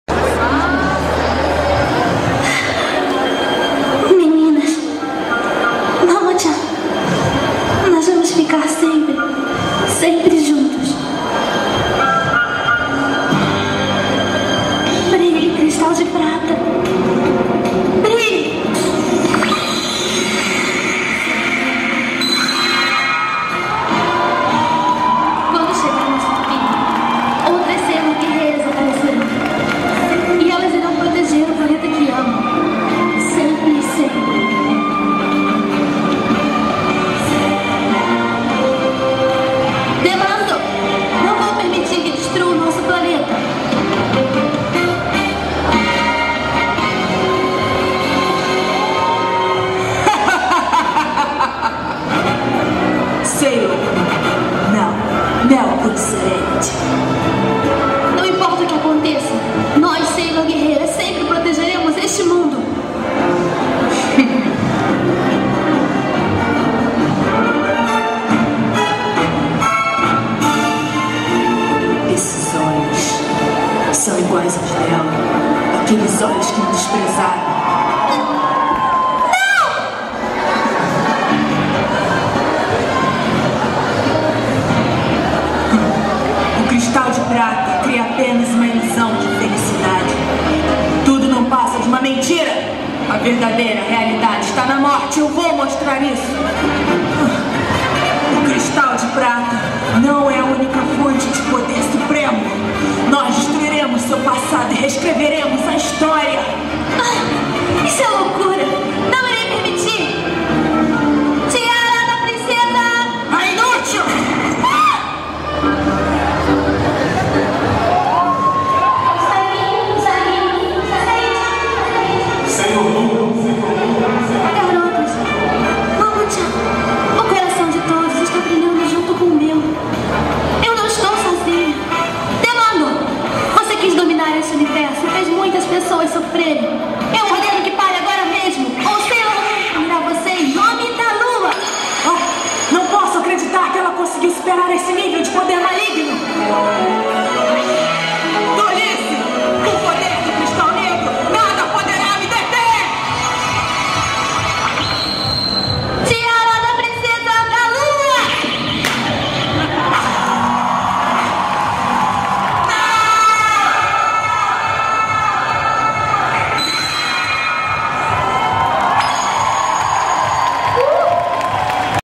WCS 2010 - Rio Anime Club - Sailor Moon
Rio Anime Club 2010
Seletiva para o WCS 2010

3o colocado
Sailor Moon